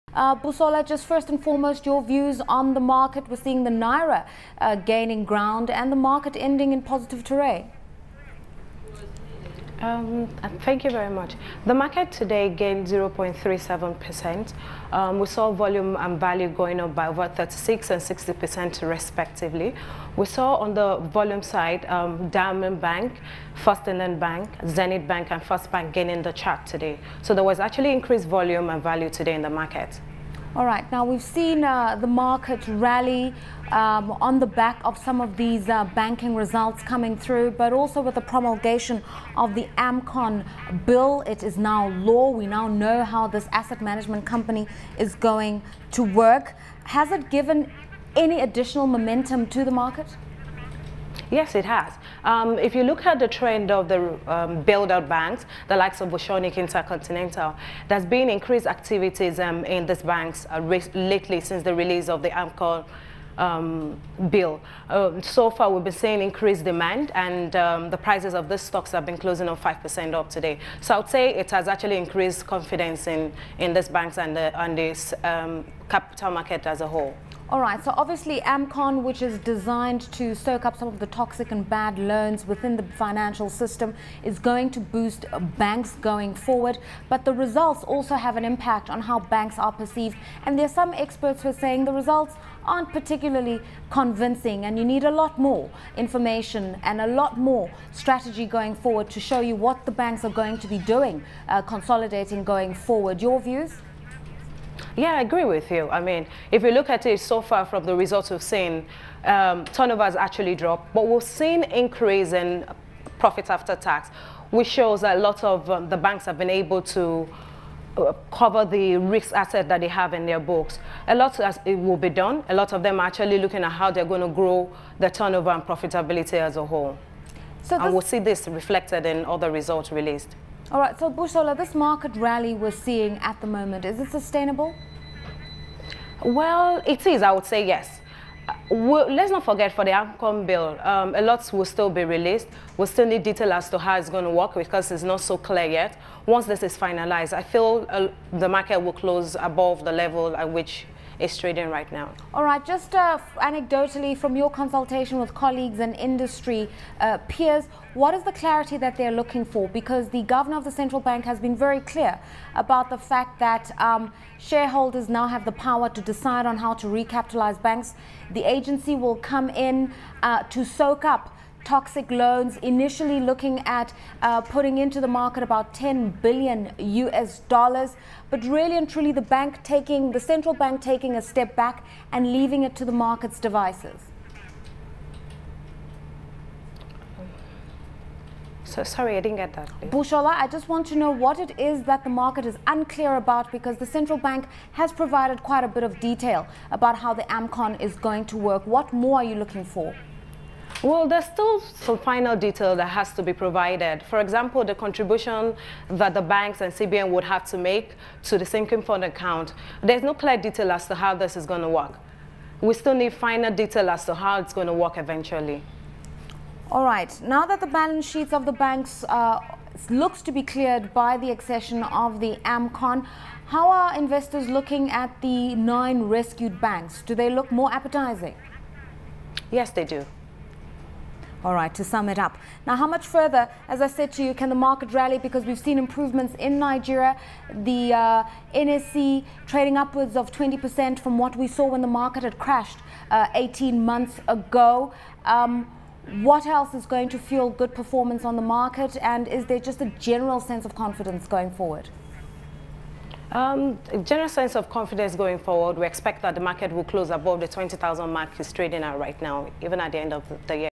22 July - Lagos Markets Wrap - Busola Jejelowo - Stanbic IBTC
Lerato Mbele speaks to Busola Jejelowo from Stanbic IBTC looking at:
Amcon Bill
Banking sector